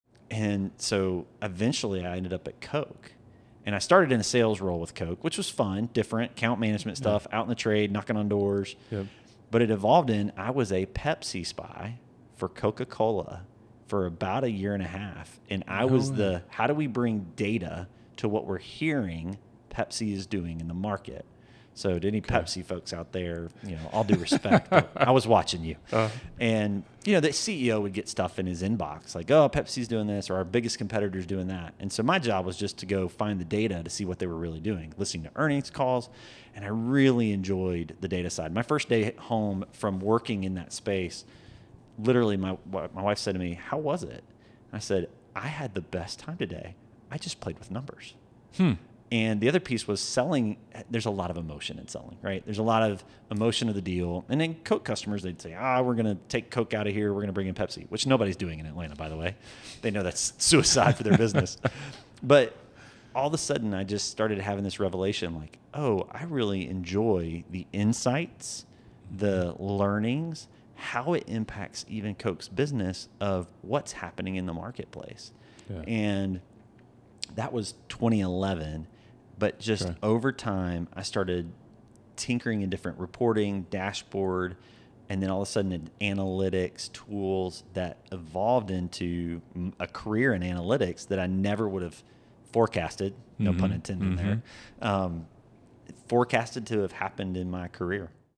Falling in Love with Data at Coke
Cooper Camak tells me how his introduction to data at Coke fueled a career